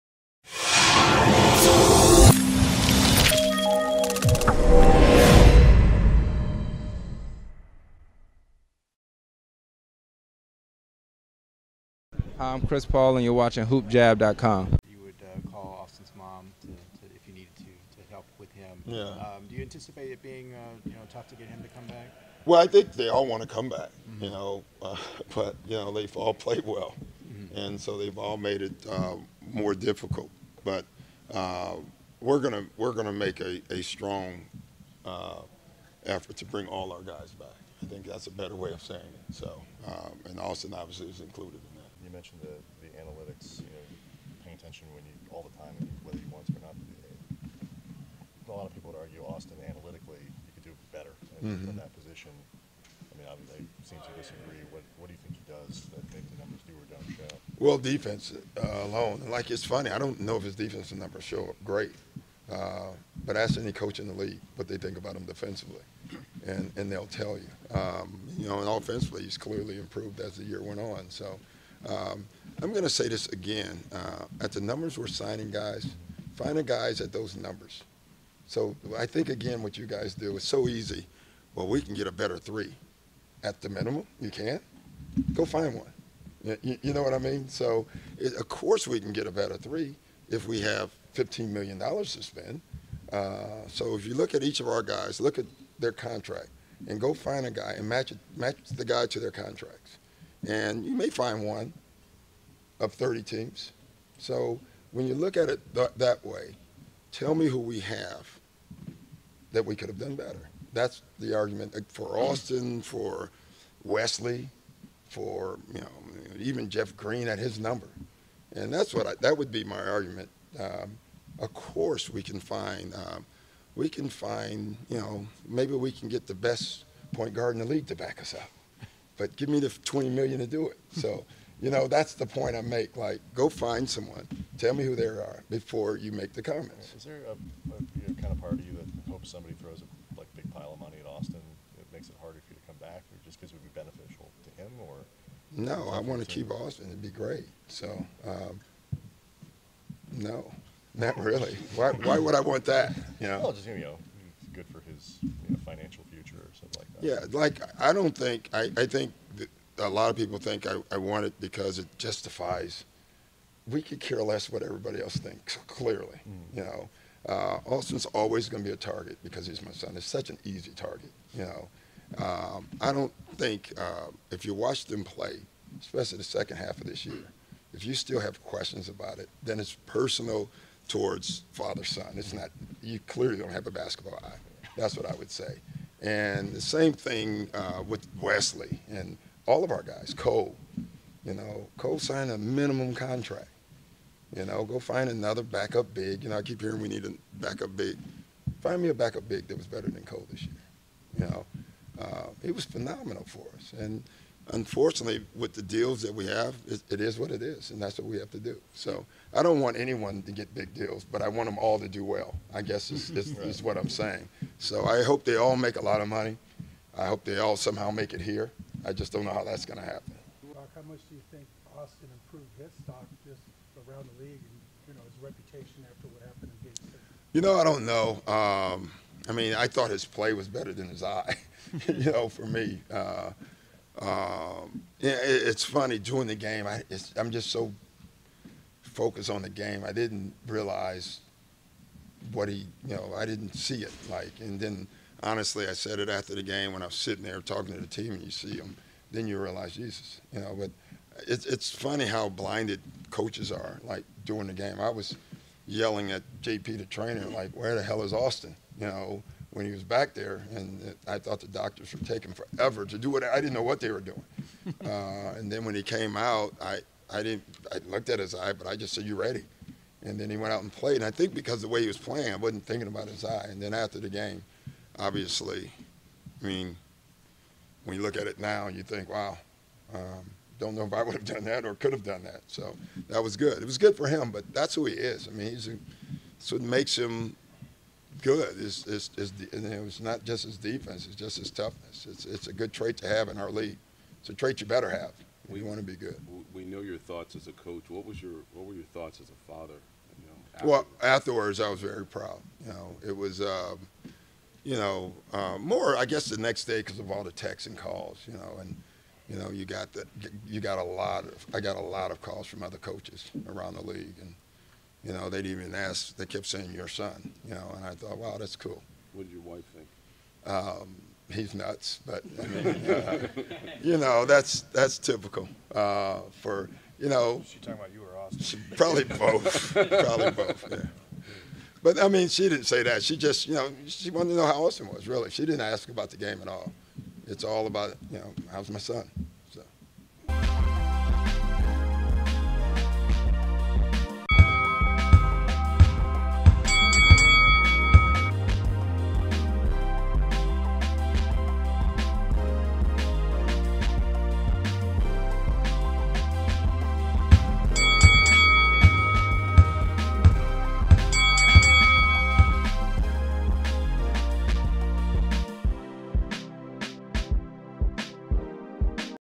Doc Rivers BLASTS Austin Rivers critics "YOU CLEARLY DON'T HAVE A BASKETBALL EYE!" HoopJab
Doc Rivers speaks about the chances of Austin Rivers returning next season, and his performance in game 6.
Dramatically increase your vertical jump!
Help HoopJab to continue providing you with great new content.